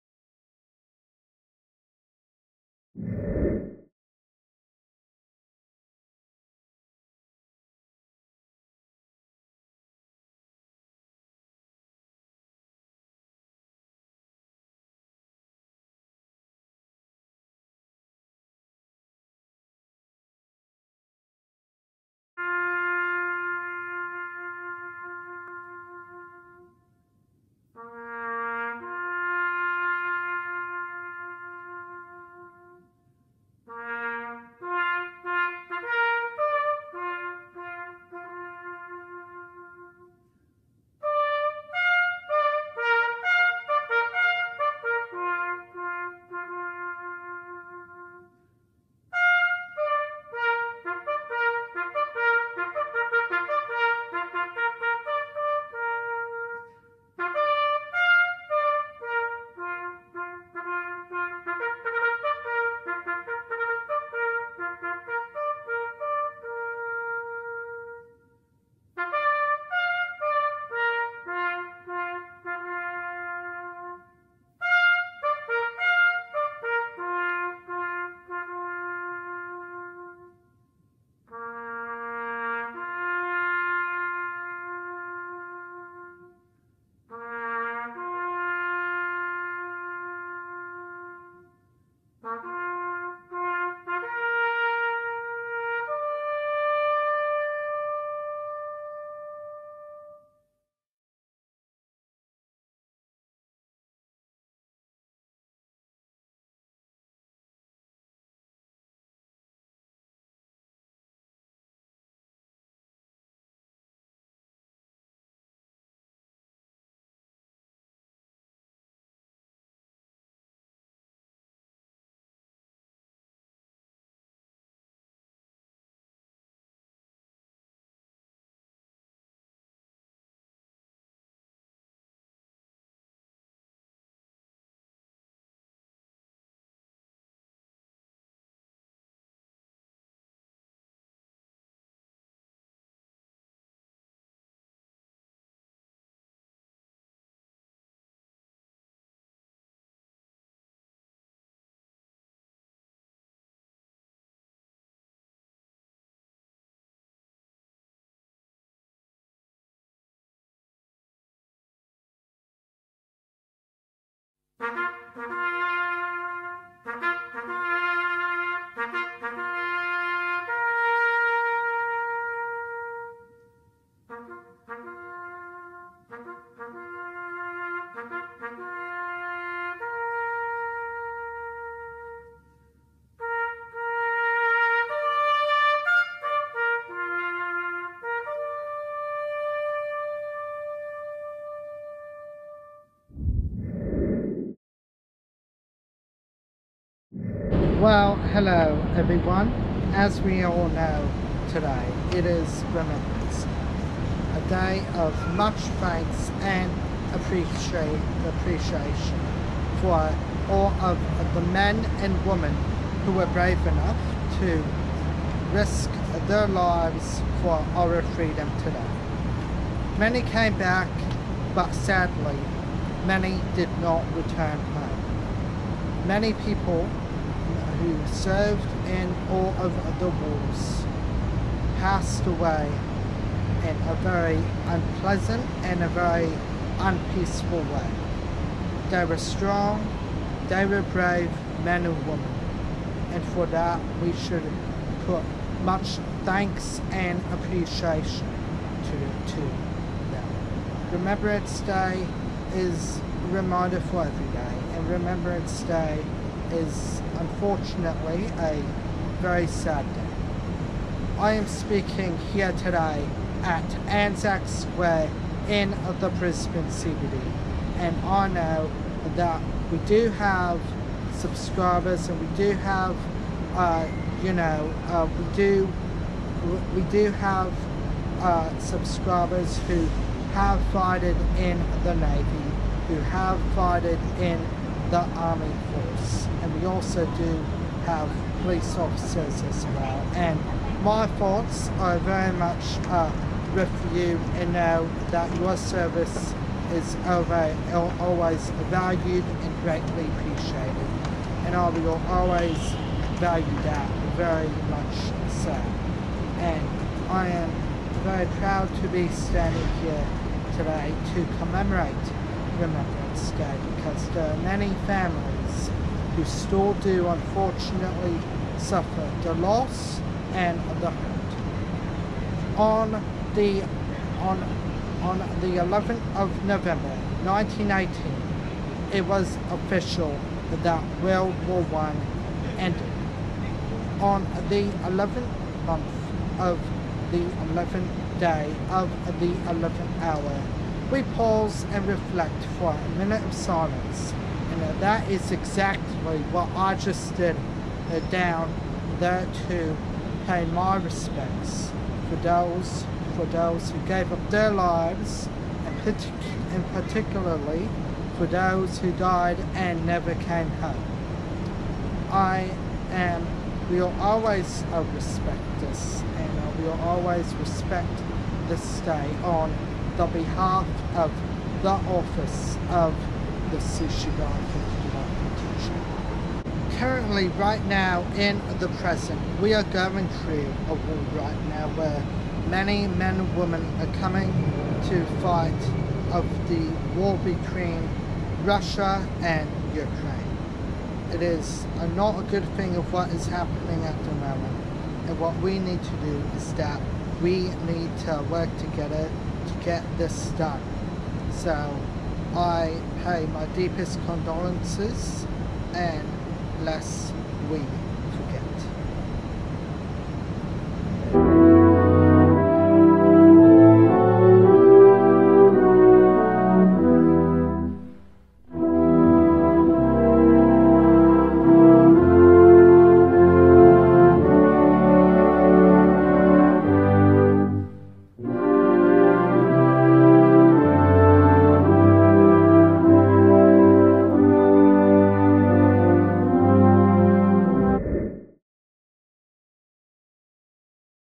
Remembrance Day 2024 - ANZAC Square
Today marks 106 Year ever since World War I ended in 1918 on the 11th of November. Today is Remembrance Day Globally, a very important day for us all to gather and pay our respects for those who gave up their lives and risked their lives for our freedom today. They were Brave Men and Women, they were strong and took a lot of Courage.